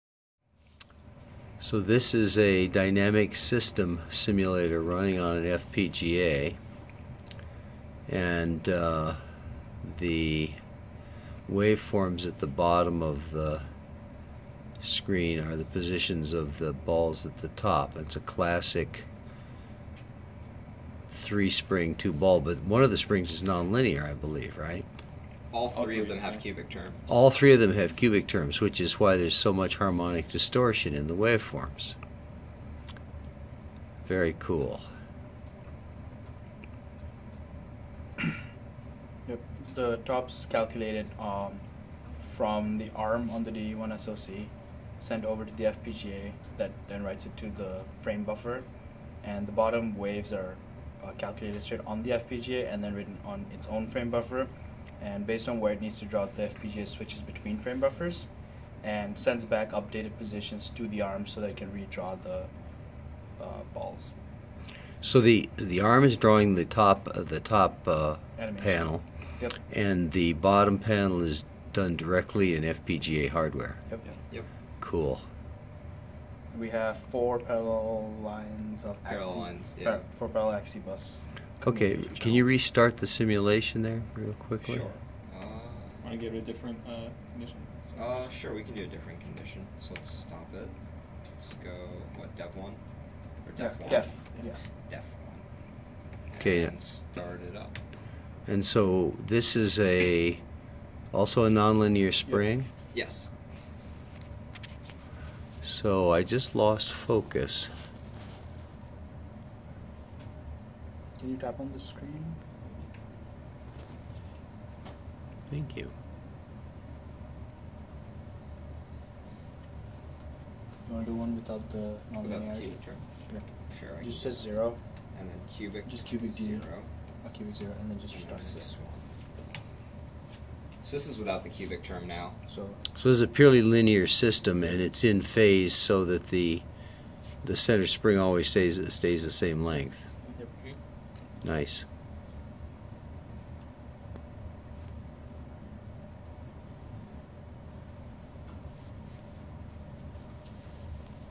Hardware ODE solver and ARM9 controller
Created by Manish Patel, Syed Tahmid Mahbub, and Matthew Filipek: We simulated a two mass, three spring oscillating system on a DE1-SoC. Euler integration was implemented in hardware to solve the differential equations, and the calculated waveforms were written to an output buffer that was read by a VGA driver to display on a VGA screen. Initial conditions were entered into a terminal on the ARM processor, and forwarded to the FPGA hardware via a parallel I/O port.
The ARM processor also received position data from the FPGA, created an animation frame that depicted the oscillation of the masses, and sent it back to the FPGA hardware so it could be displayed at the top of the screen. Non-linear effects could be modeled by enabling a cubic force term on the springs in the system.

The DE1-SoC board FPGA was used to make a parallel ODE solver controlled by the ARM CPU. Lab description
Project by by Tahmid Mahbub, Manish Patel, Matt Filipek